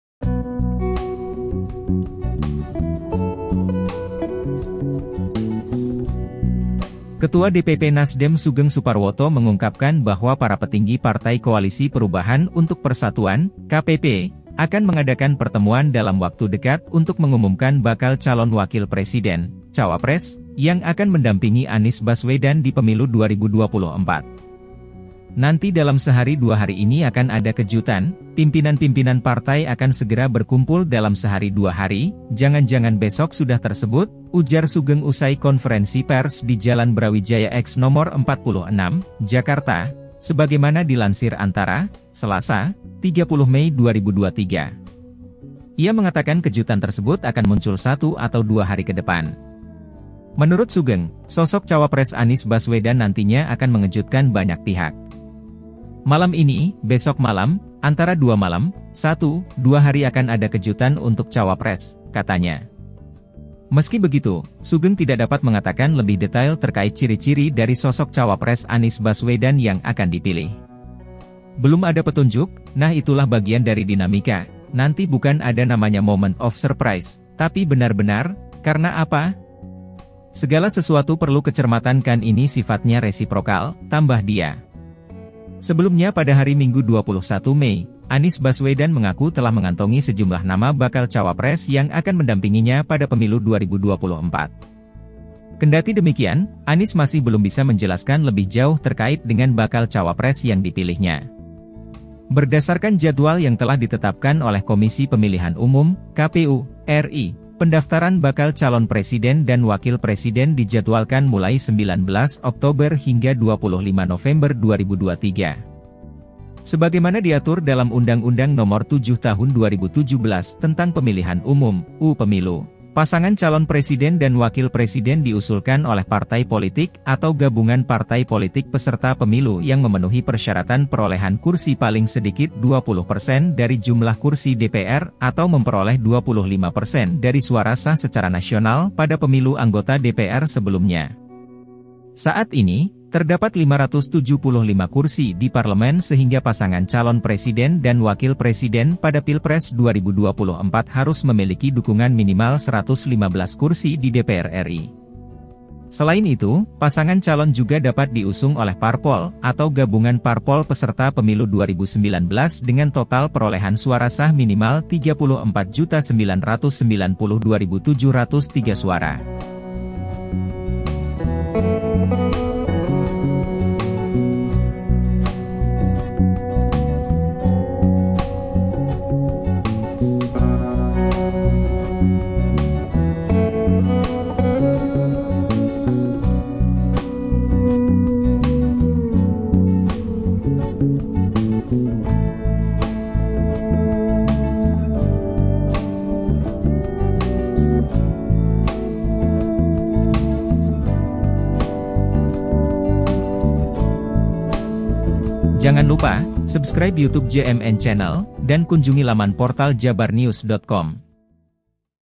Ketua DPPP NasDem ungkap sosok cawapres Anies Baswedan segera diumumkan
Ketua DPP NasDem Sugeng Suparwoto mengungkapkan bahwa para petinggi partai Koalisi Perubahan untuk Persatuan (KPP) akan mengadakan pertemuan dalam waktu dekat untuk mengumumkan bakal calon wakil presiden (cawapres) yang akan mendampingi Anies Baswedan di Pemilu 2024.

"Nanti dalam sehari dua hari ini akan ada kejutan, pimpinan-pimpinan partai akan segera berkumpul dalam sehari dua hari, jangan-jangan besok sudah tersebut," ujar Sugeng usai konferensi pers di Jalan Brawijaya X Nomor 46, Jakarta, sebagaimana dilansir Antara, Selasa (30/5/2023).

Ia mengatakan kejutan tersebut akan muncul satu atau dua hari ke depan.

Menurut Sugeng, sosok cawapres Anies Baswedan nantinya akan mengejutkan banyak pihak.

"Malam ini, besok malam, antara dua malam, satu, dua hari akan ada kejutan untuk cawapres," katanya.

Mungkin beberapa footage di channel ini bukan milik kami,
Kami hanya berusaha membuat video berdasarkan aturan penggunaan wajar
untuk pemberitaan, pembelajaran serta komentar.